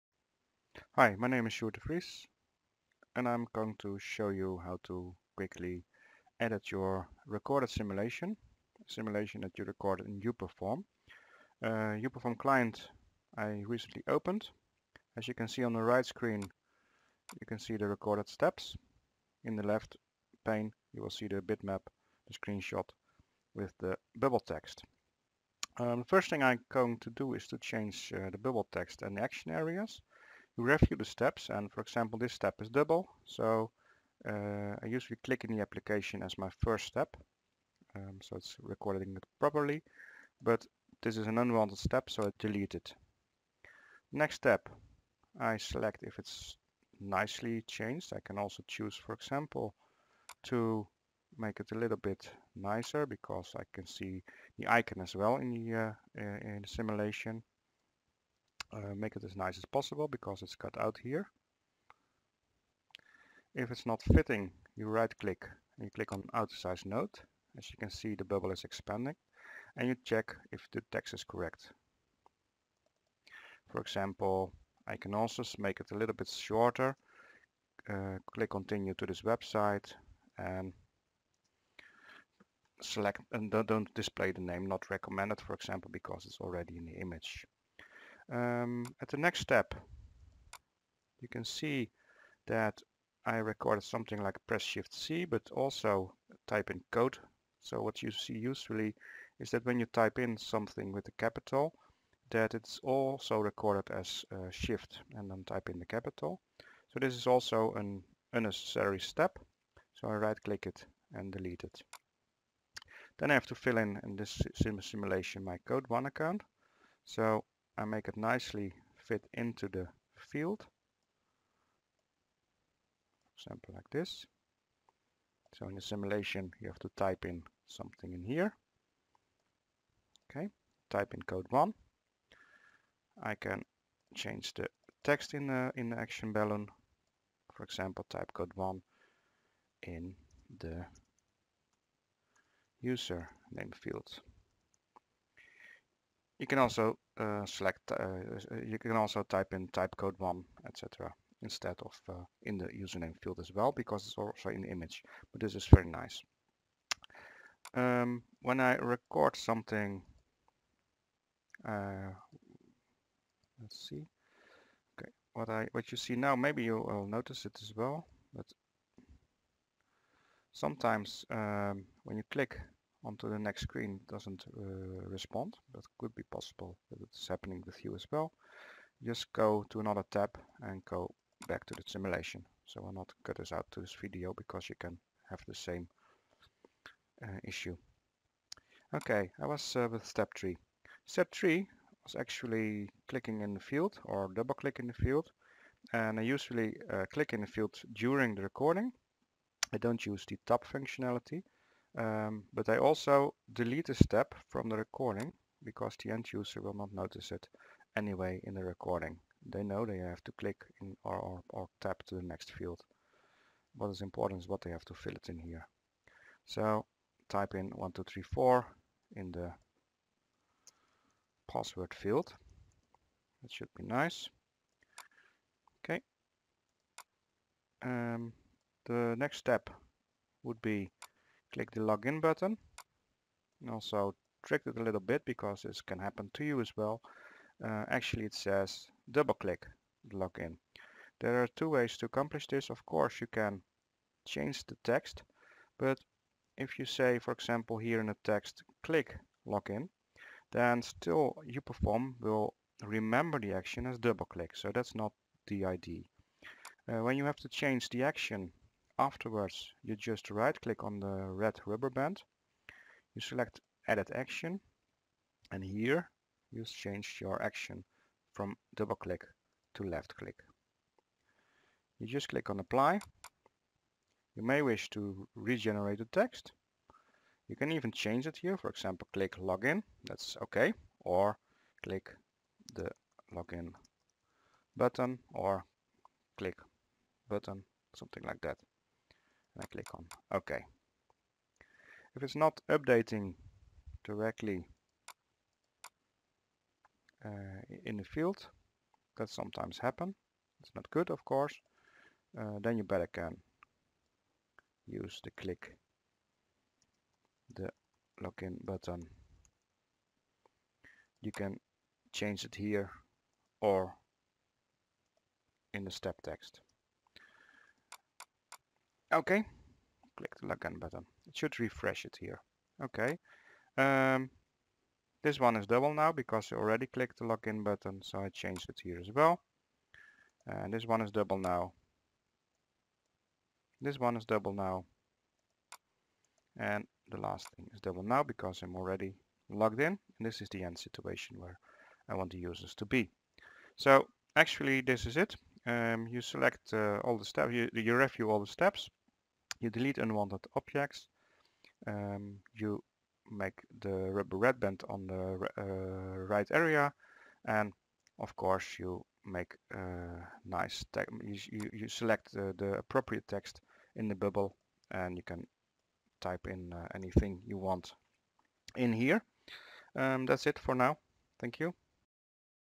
Edit a simulation in uPerform
Tutorial on how to edit a simulation in uPerform.